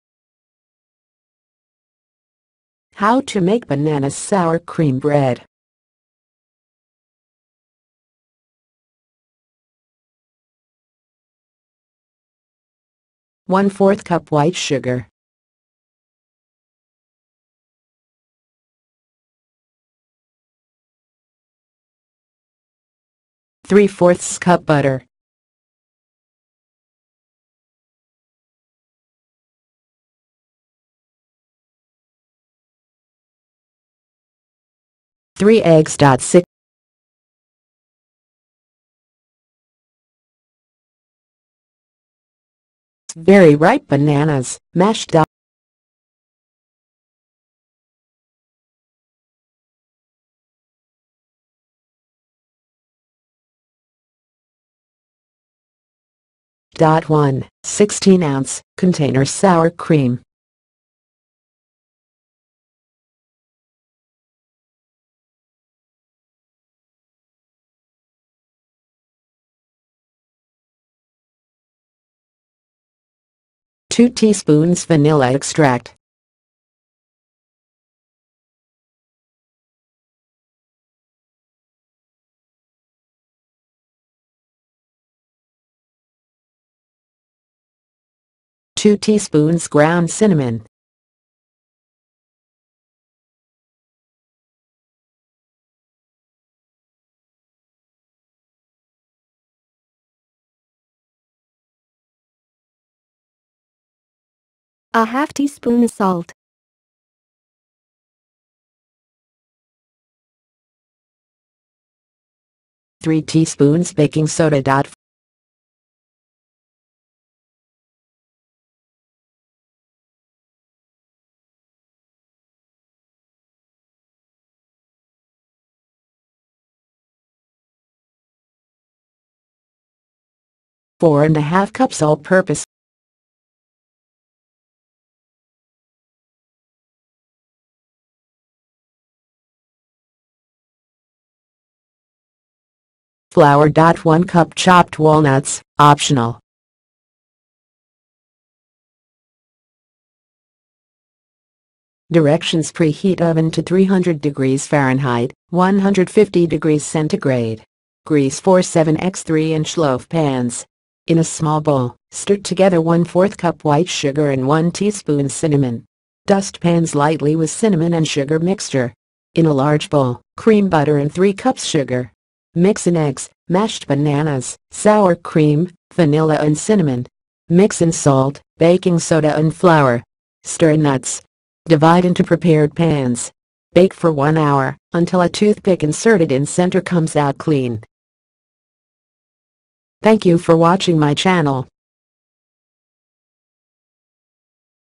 How To Make Banana Sour Cream Bread Upload 2017
"I know, you're probably thinking, 'Oh no! Another banana bread recipe!' But this one is a little different: the sour cream makes this one so moist it melts in your mouth. The flavor is just wonderful! This one is great for gift giving and holidays. Loaves freeze well."